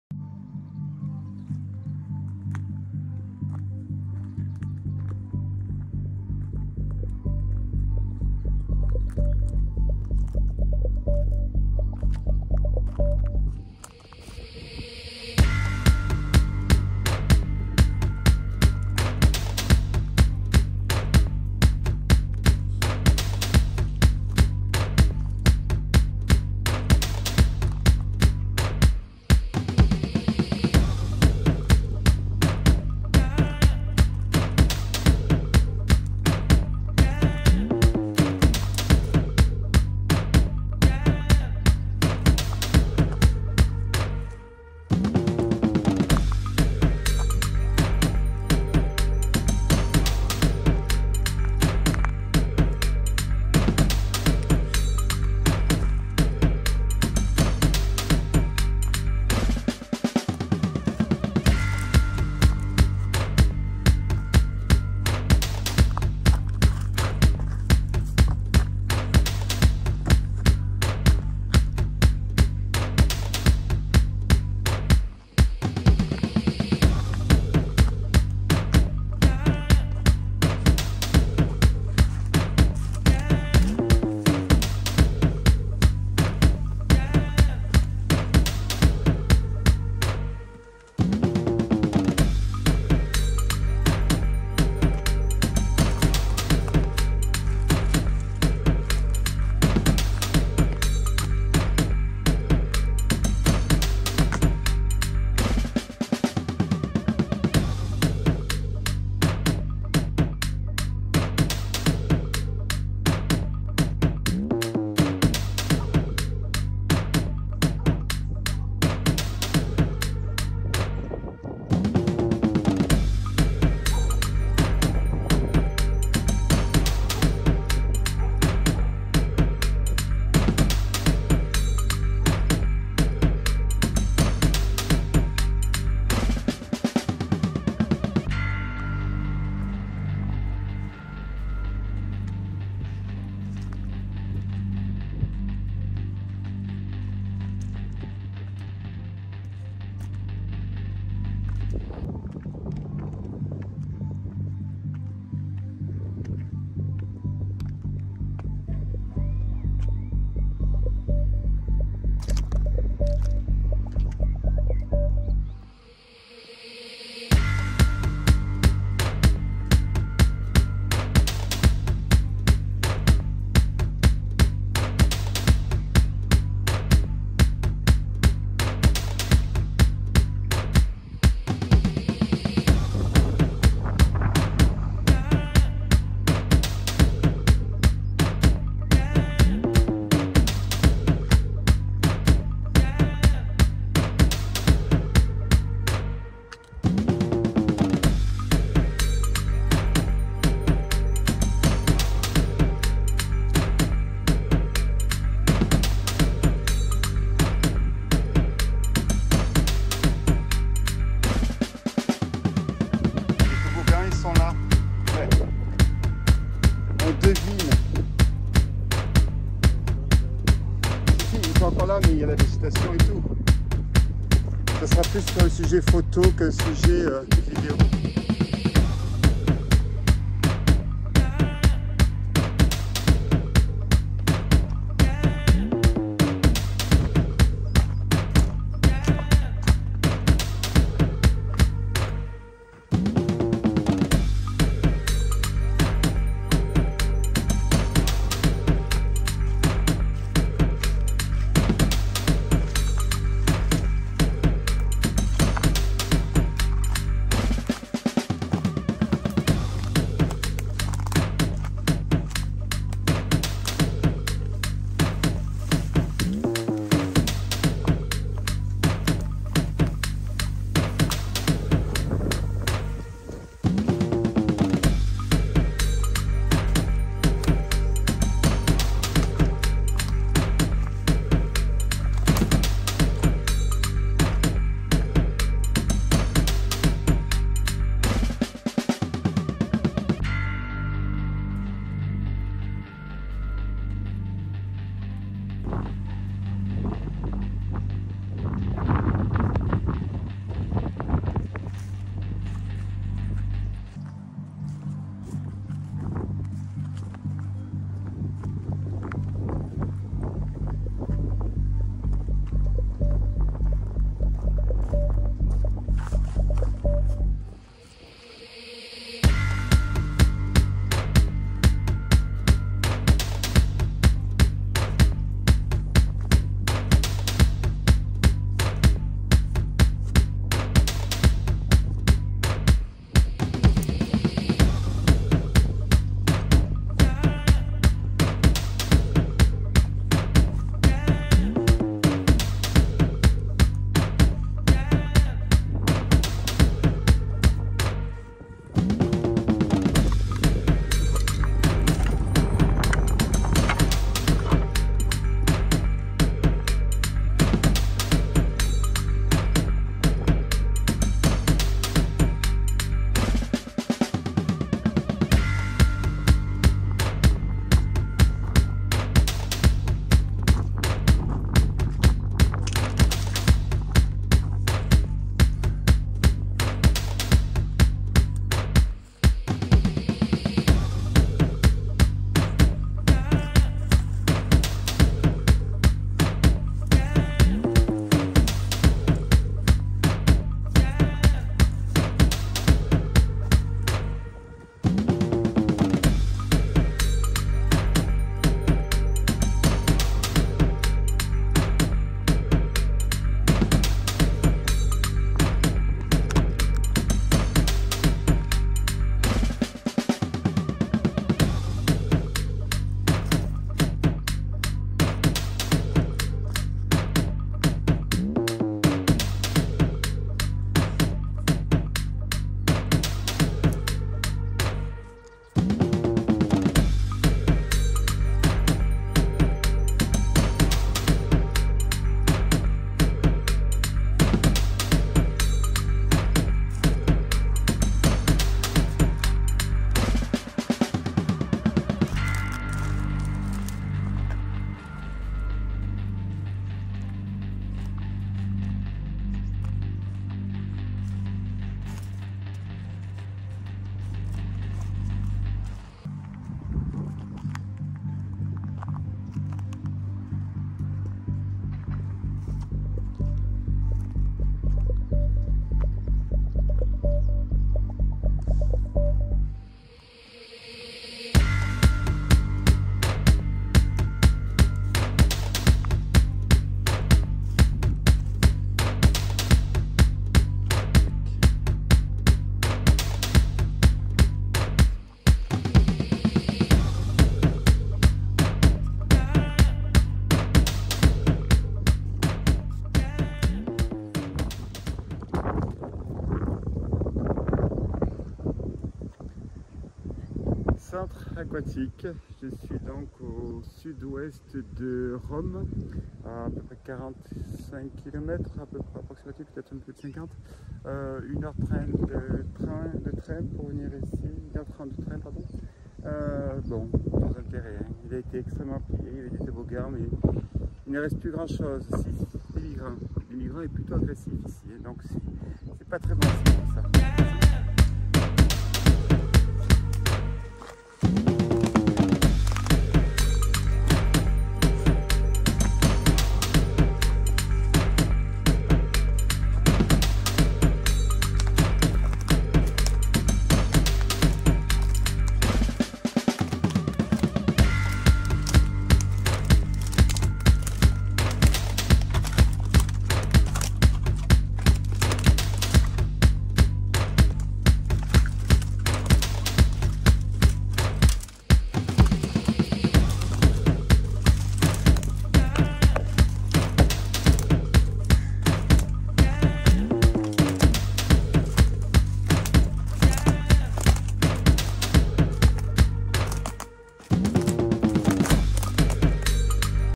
URBEX Guy Clément ITALIA | PESCARA | AQUATIC PARK VIDEO
Une exploration en Italie, au sud ouest de Rome. Cet ancien parque aquatique, abandonnée depuis longtemps, vient de voir ses trois tobogans disparaitre à jamais...
An exploration in Italy, southwest of Rome. This former aquatic park, abandoned for a long time, has just seen its three slides disappear forever...
Исследование в Италии, к юго-западу от Рима. Этот бывший водный парк, заброшенный на долгое время, только что увидел, как его три горки исчезают навсегда...
Eine Erkundung in Italien, südwestlich von Rom. Dieser alte, lange verlassene Wasserparque sah seine drei Tobogans für immer verschwinden...
Badanie we Włoszech, na południowy zachód od Rzymu. Ten dawny park wodny, porzucony przez długi czas, właśnie widział, że jego trzy zjeżdżalnie znikają na zawsze...
Una exploración en Italia, al suroeste de Roma. Este antiguo parque acuático, abandonado desde hace mucho tiempo, acaba de ver desaparecer para siempre sus tres toboganes...
Un'esplorazione in Italia, a sud-ovest di Roma. Questo antico parco acquatico, abbandonato da tempo, ha appena visto i suoi tre scivoli scomparire per sempre...